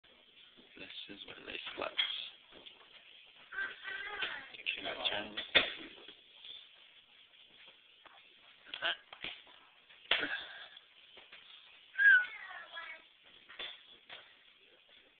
Old Town San Diego Tour Part 5
*I monitor all comments*
My Dad, some family out of state and I went on a Old Town San Dieo Tour, (it also went to other places). I am walking to where some of the crew had slept.